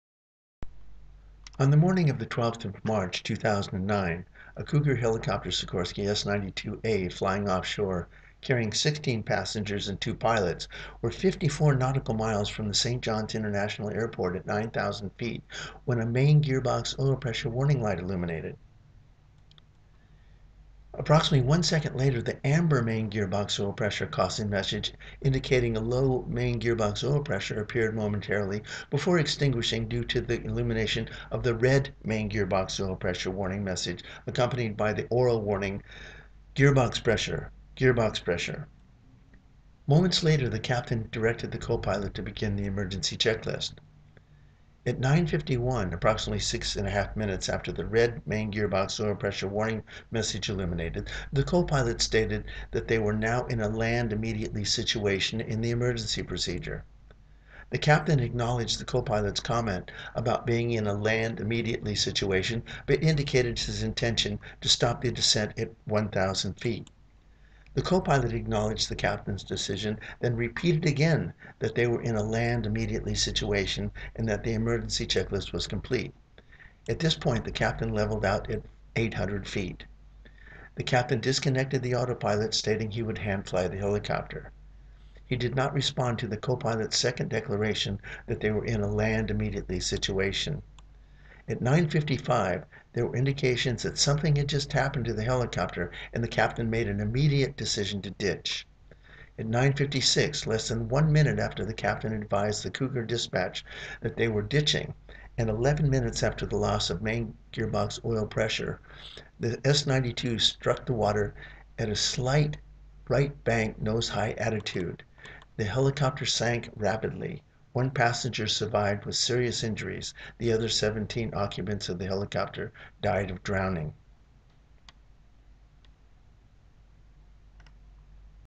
Cougar S 92 crash
Computer generation by Transport Canada of Cougar Helicopters Flight 91.  I have done a voice over taken from the TSB investigation report of the crash.  This was a scheduled flight of a Cougar Sikorsky S-92A which ditched on 12 March 2009 en route to the SeaRose FPSO in the White Rose oil field and Hibernia Platform in the Hibernia oilfield off the coast of Newfoundland 55 kilometres (34 mi) east-southeast of St. John's, Newfoundland.